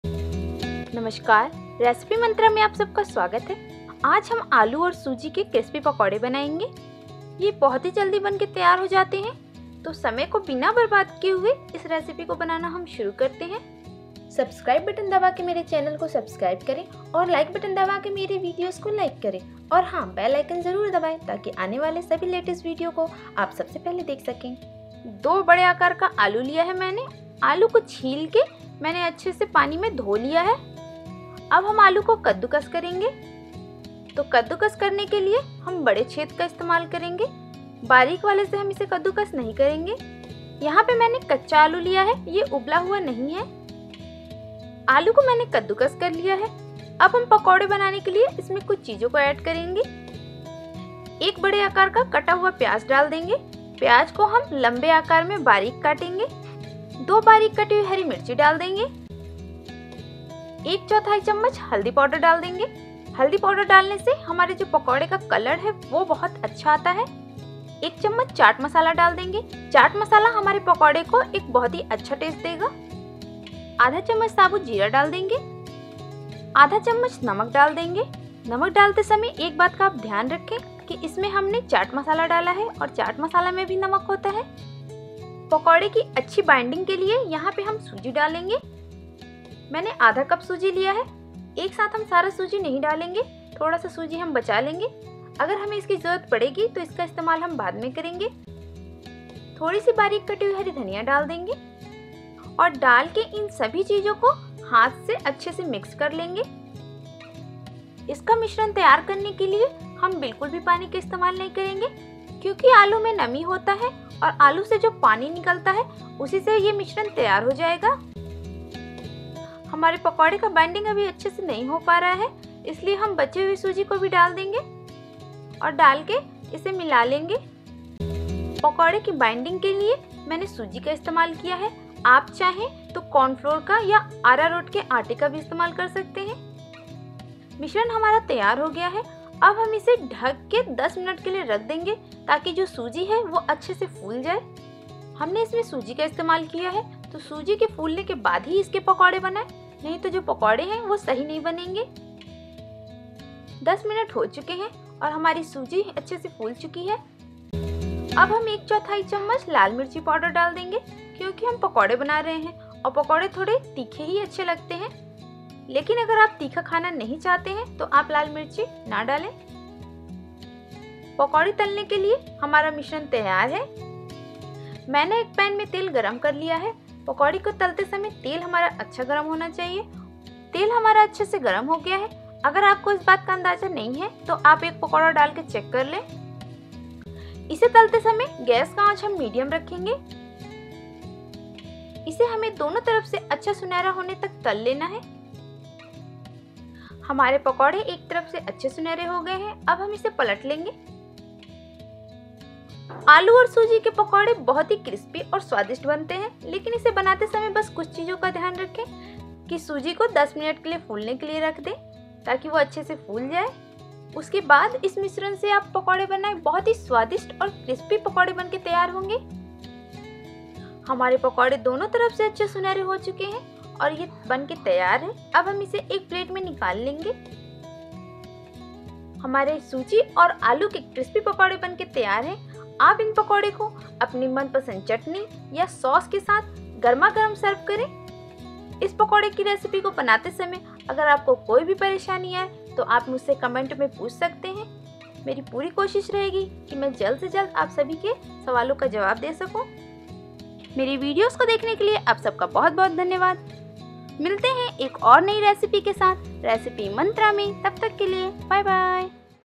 Crispy Aloo Suji Pakore Recipe | आलू सूजी के लच्छेदार कुरकुरे पकोड़े जो आपने कभी नही खाये होगे
बारिश के मौसम में पकोड़ी का अपना ही एक अलग मज़ा है। पकोड़ी कई प्रकार से बनाई जाती है। आज हम आलू और सूजी के लच्छेदार कुरकुरे पकोड़े जो आपने कभी नही खाये होगे | ये खाने में बहुत ही क्रिस्पी लगते है और बहुत ही आसानी से बनकर तैयार भी हो जाते है। आइये जाने आलू सूजी के पकोड़े बनाने की विधि।

Thank you so much for lovely comment and watching Video.